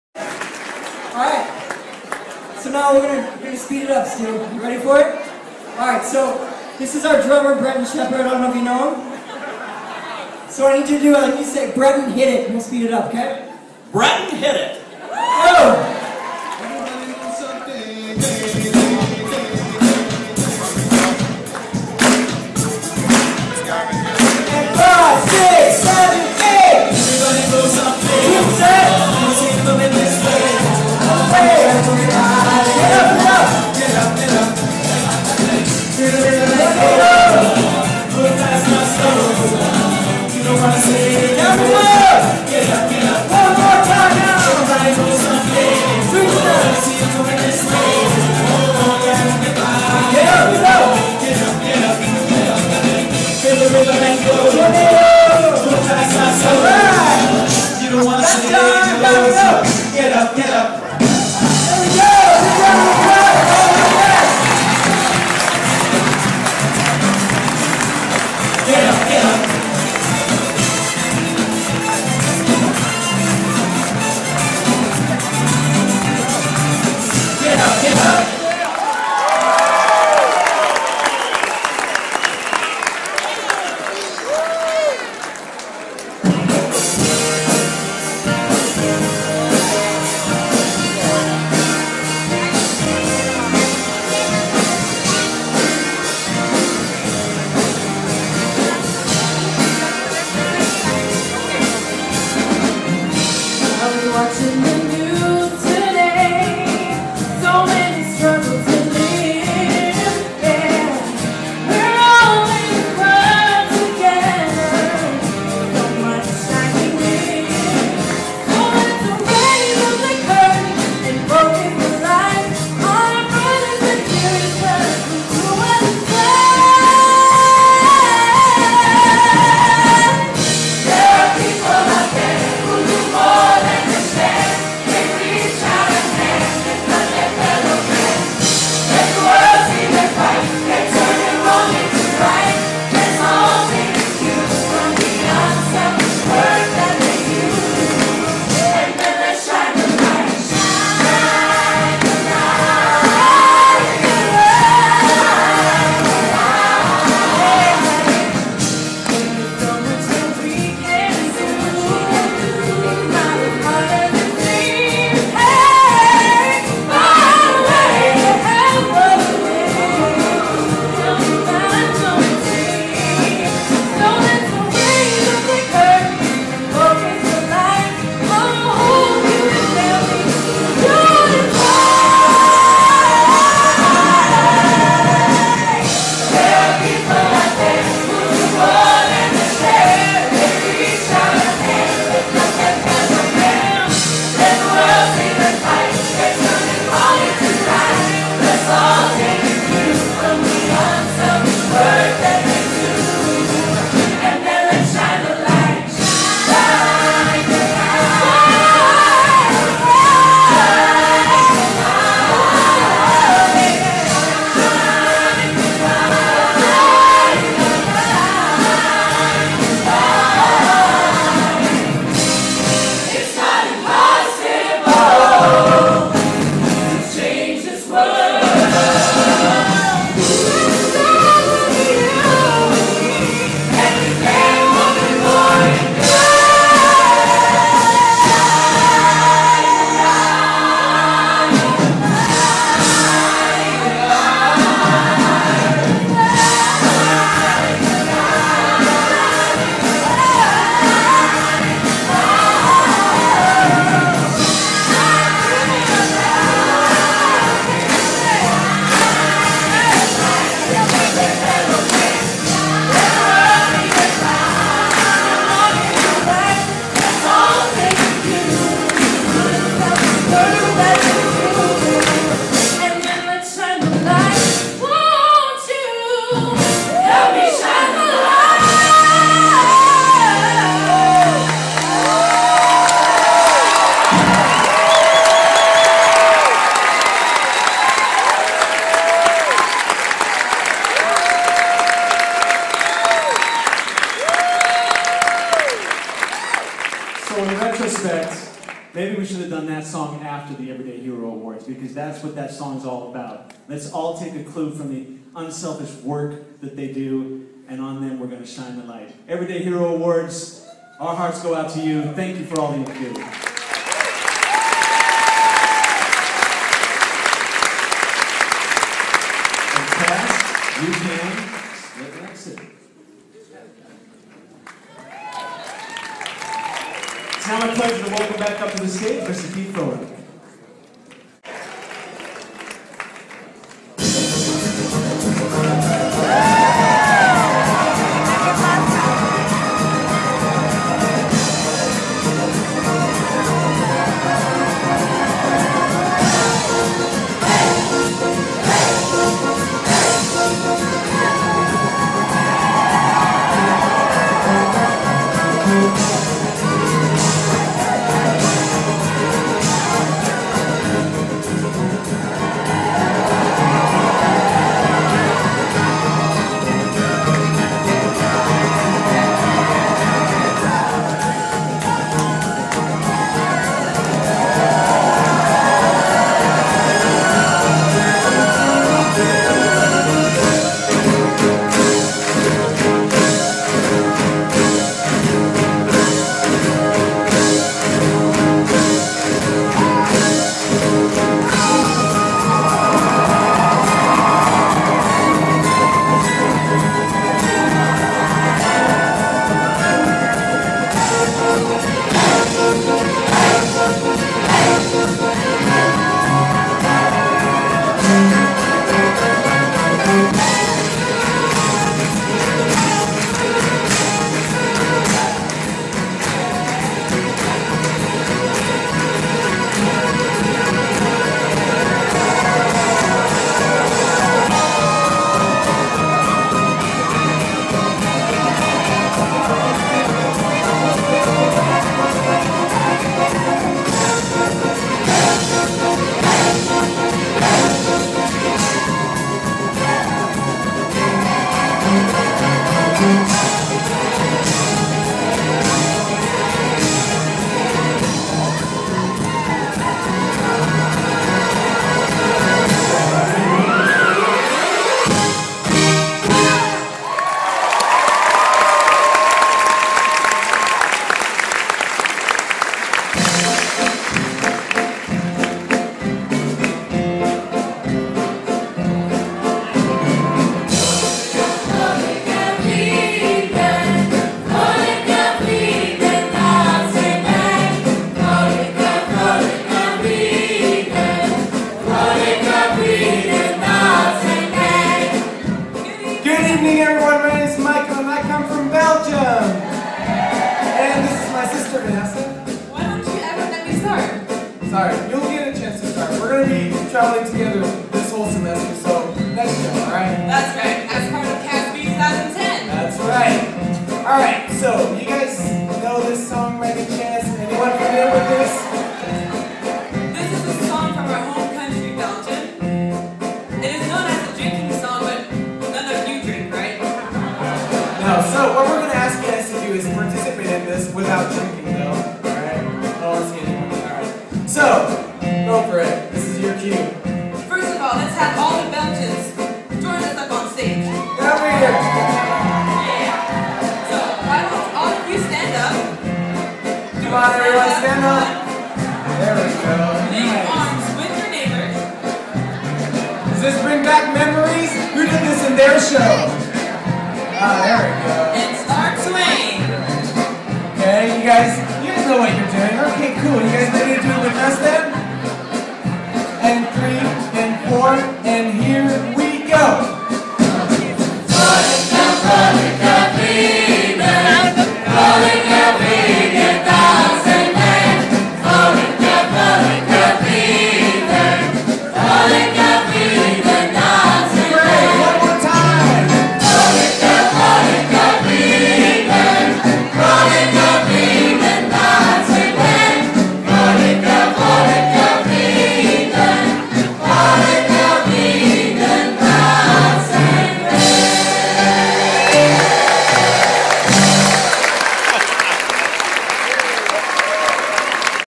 2010 UWP B Part 5
REUNION 2010 Cast B's performance at Reunion 2010, Part 5. This cast was only half way through summer staging at the time of this performance for alumni.